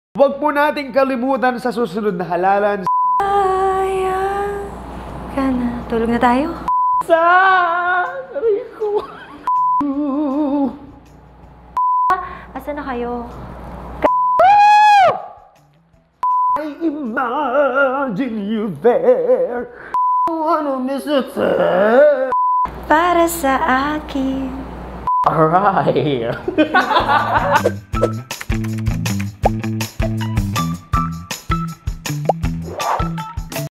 IBA’T IBANG URI NG SINGER SA PILIPINAS
Basta kantahan, Pinoy ang nangunguna diyan!

Alamin ang iba’t ibang uri ng Singer sa Pilipinas 🎶 🇵🇭

Abangan ang full video bukas!